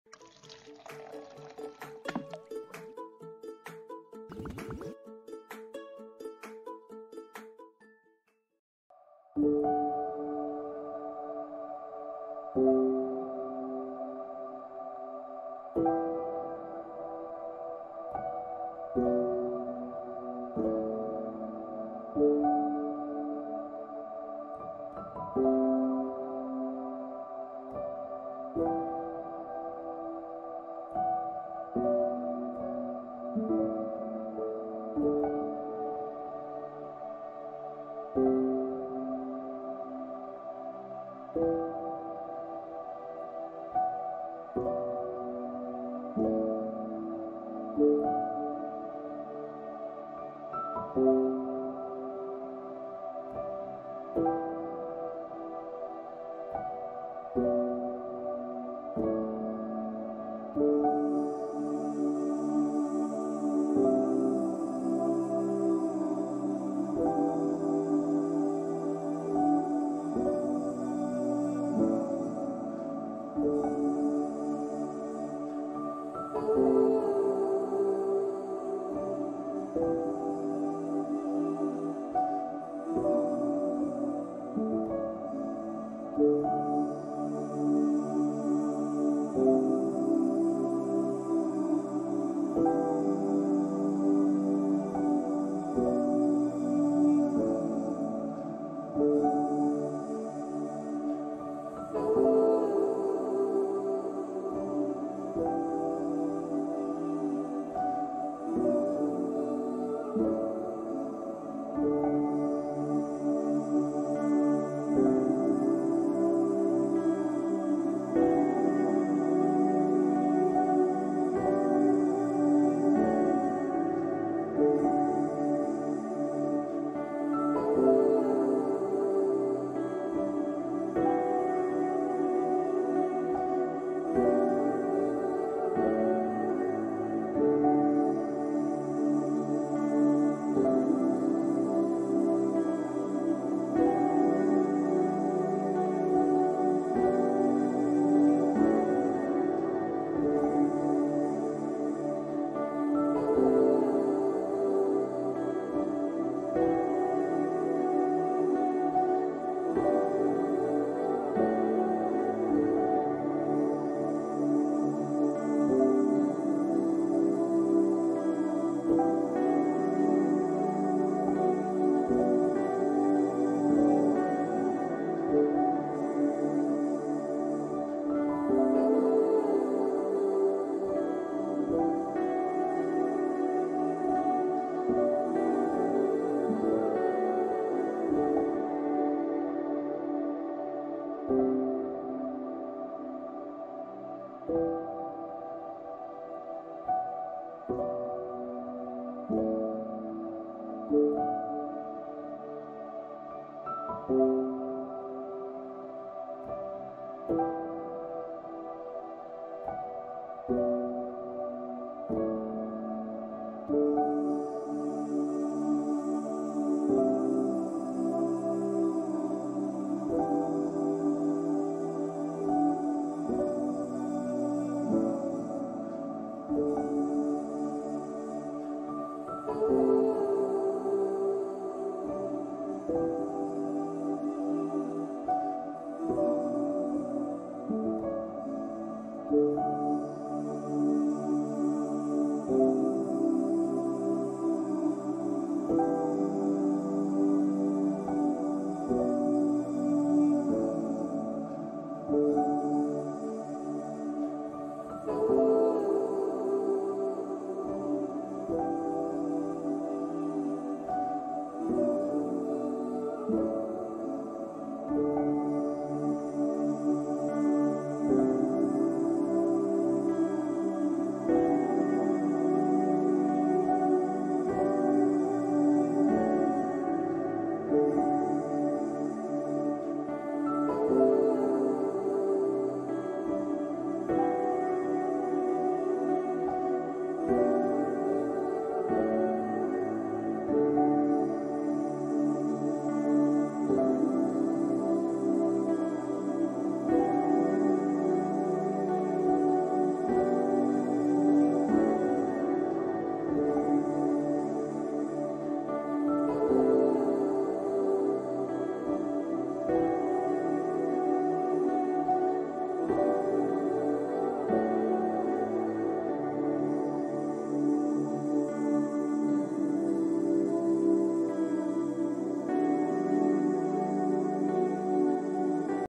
Jellyfish Tank - Jellyfish Aquarium For Relaxation
Relax and check out the variety of invertebrates in our jellyfish tank. Footage for jellyfish aquarium for relaxation from our awesome oceans. Stunning jellyfish 4k footage.